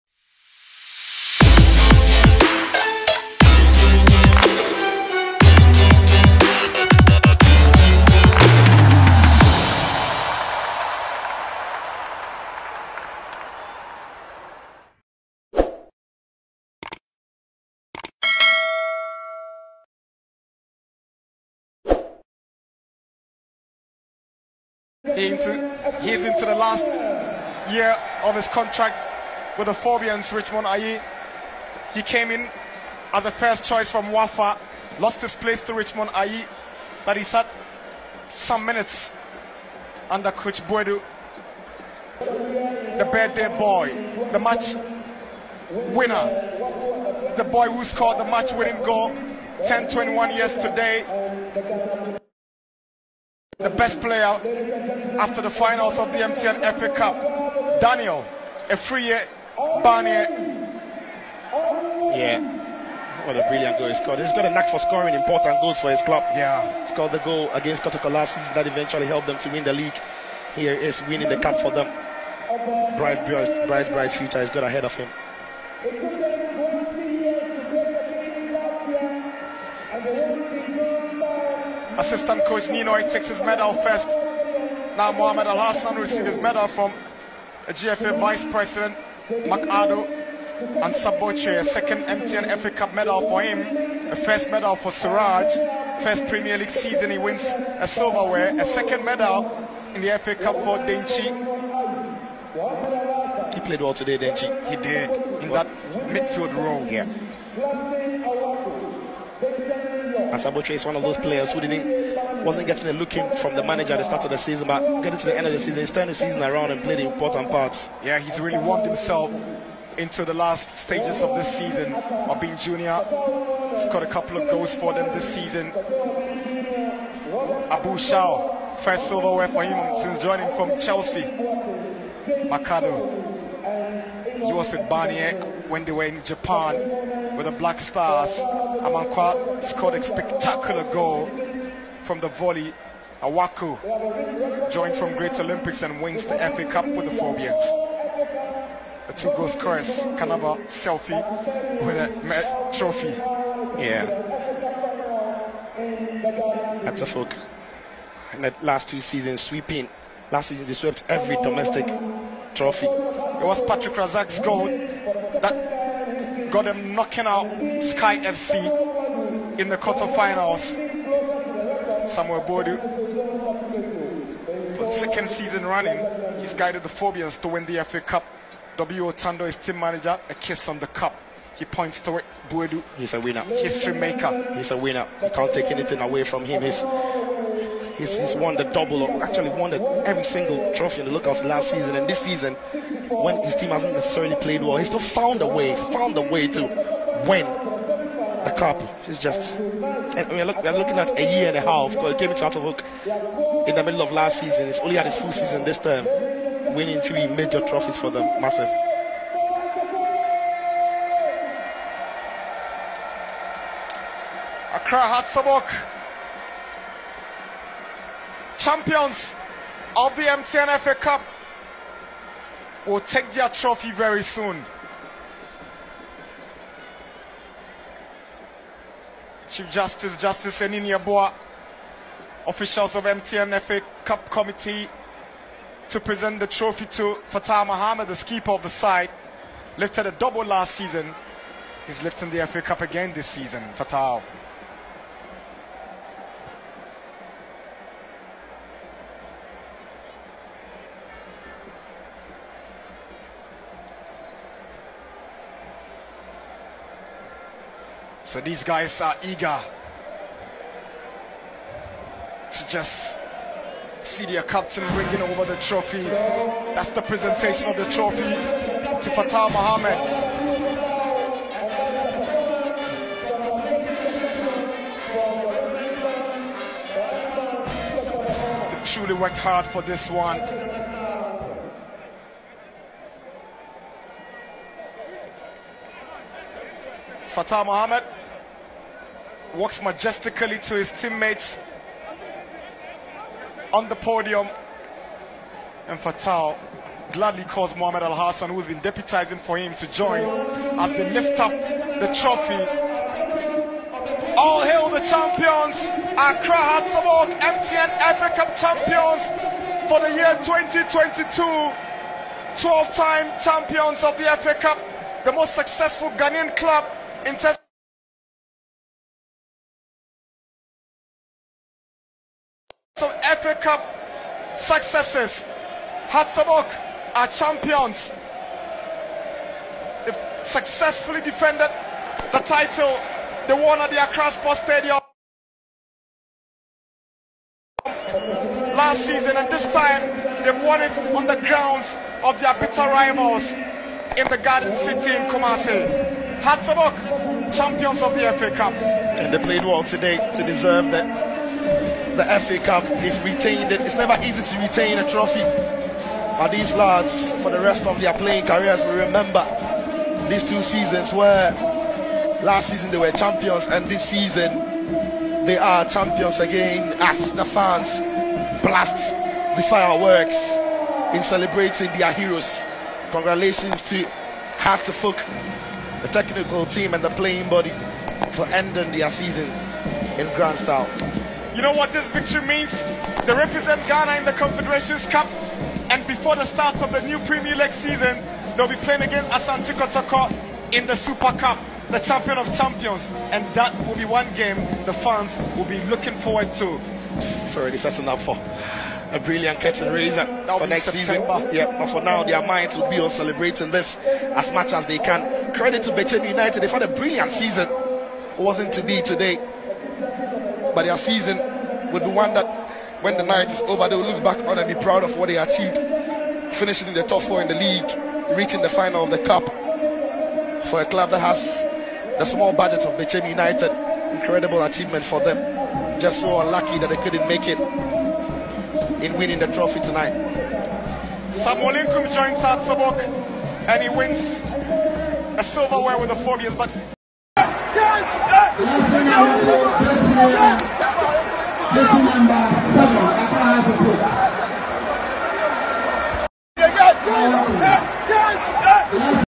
Hearts Of Oak Massice Celebrations After FA Cup Win🔥, Serious Scenes, Afriyie Barnie, Coach Boadu
Watch Hearts Of Oak Massive Celebrations after MTN FA CUP WIN AT BABA YARA SPORTS STADIUM, Afriyie Barnie, Coach Samuel Boadu, Gladson Awako, Hearts Of Oak Supporters.